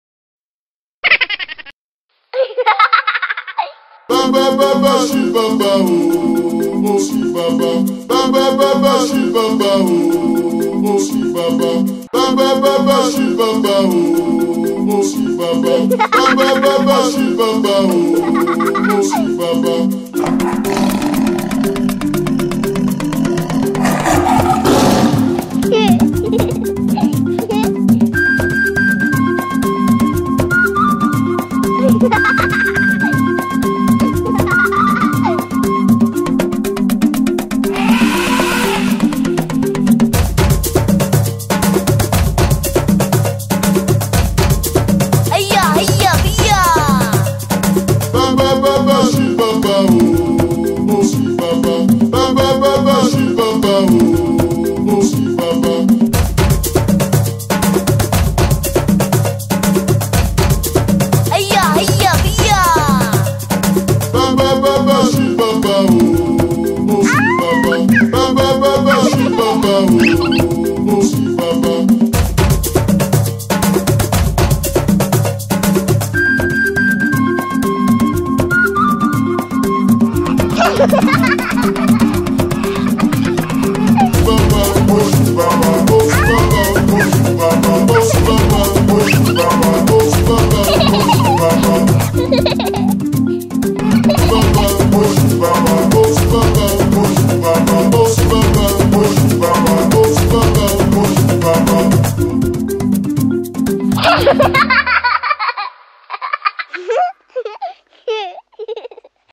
Baby Dance Cartoon Video Jungle Edition | Infobells
This fun Baby Dance cartoon video along with animals is delivered with full of energy and movement, designed to entertain you and your little one.